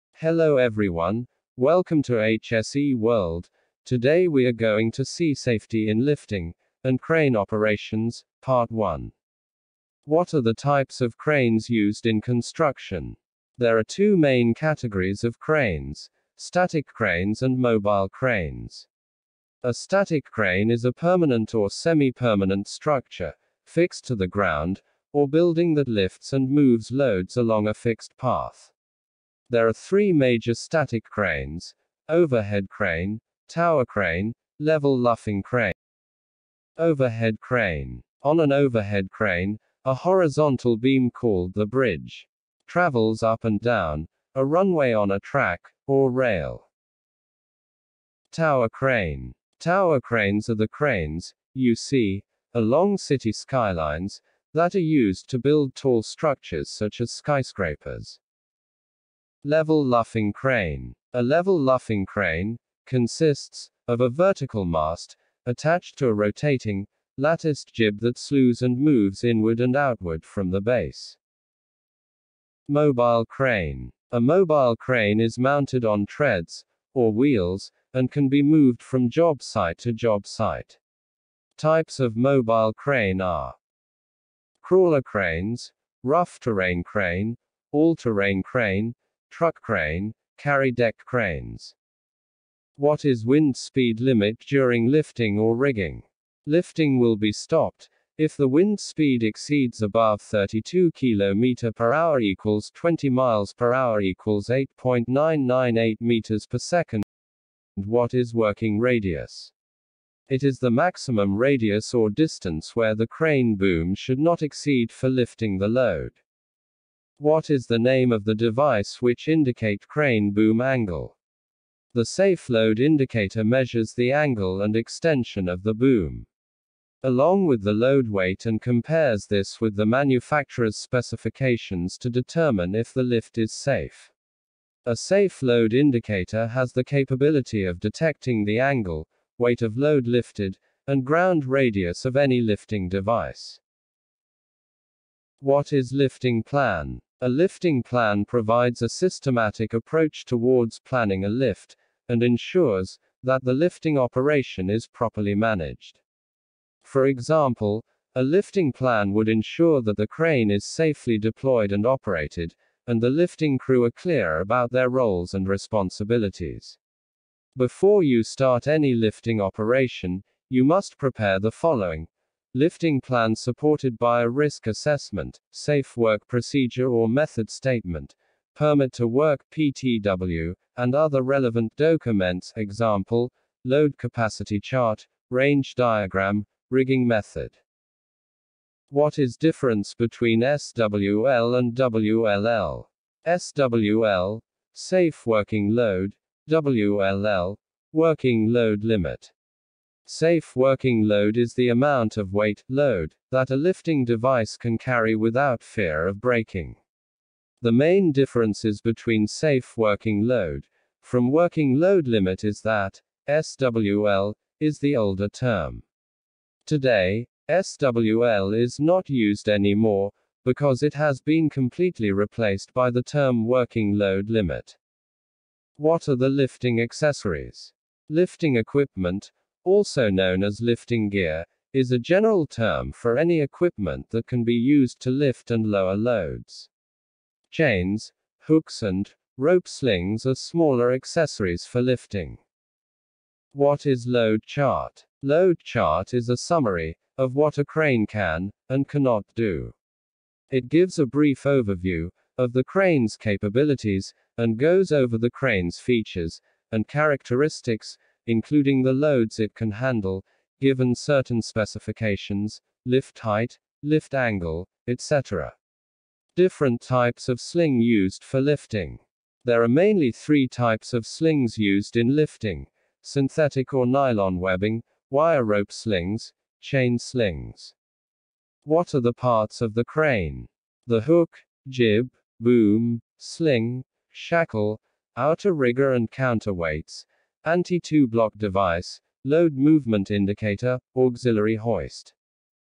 Crane Lifting Safety Interview Question and Answer part -1
Crane lifting safety interview question and answer part -1 | Hse world| Lifting safety question and answer#safety

In this video we will see the safety in crane lifting operations. The risk and hazard and preventive control while performing crane lifting activities. And will see the types of crane and parts of crane in construction and oil and gas site. leave your feedback .

Checkout our latest video on fire safety